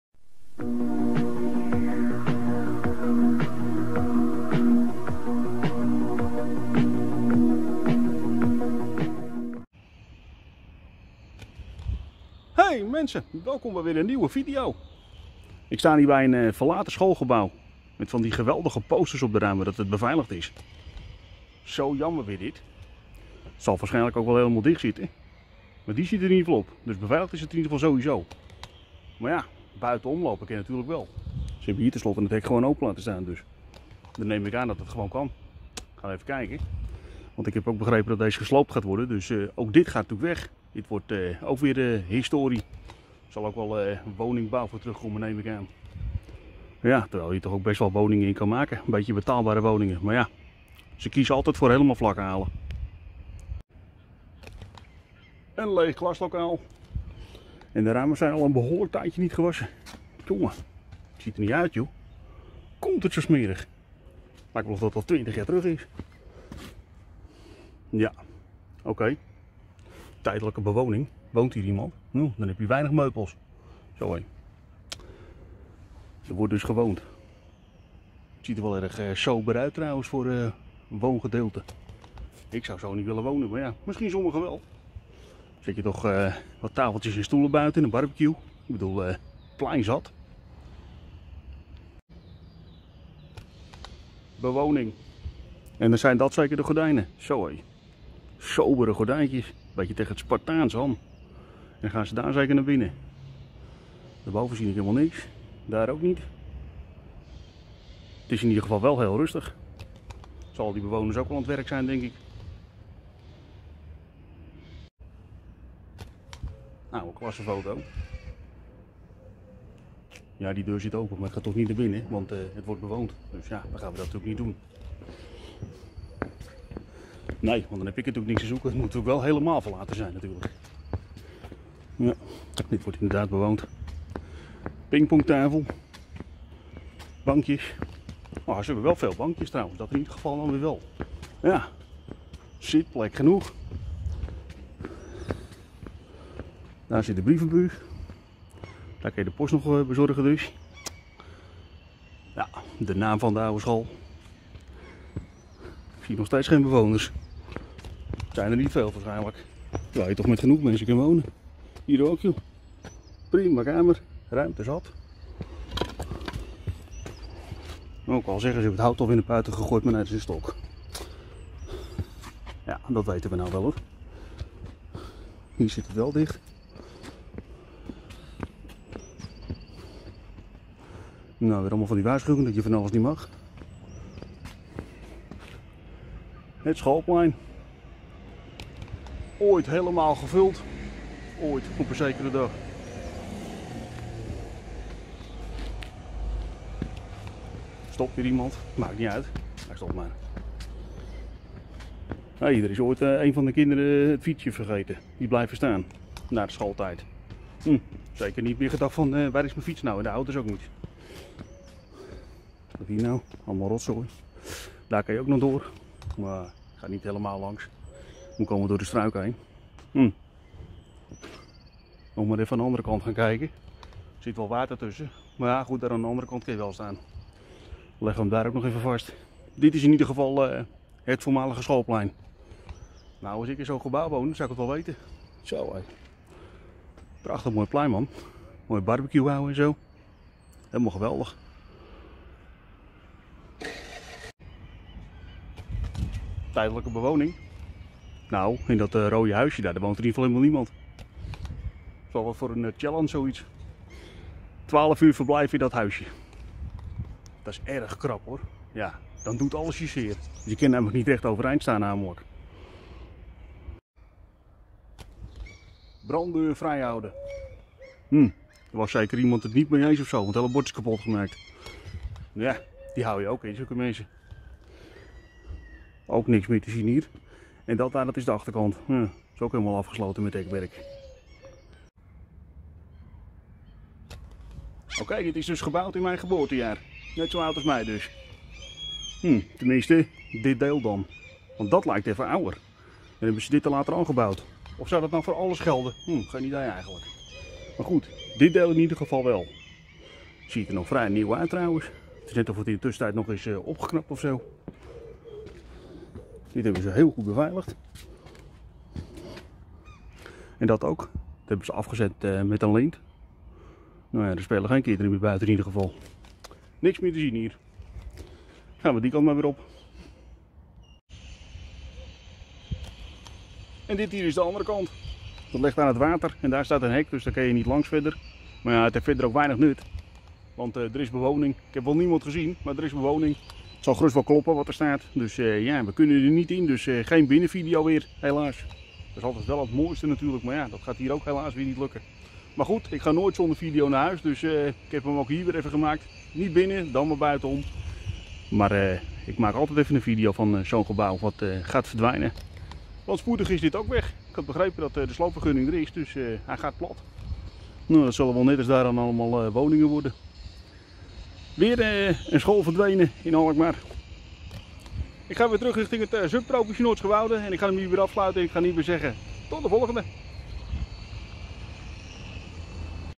Verlaten school helaas bewaakt door tijdelijke bewoning
In deze video neem ik een kijkje rond een verlaten schoolgebouw in Alkmaar. Helaas is er niet in te komen omdat het bewaakt word door tijdelijke bewoning. Dat is jammer want een binnenvideo is het mooiste maar ik ga nooit zonder video naar huis dus dan maar eentje buitenom. Ook deze school gaat verdwijnen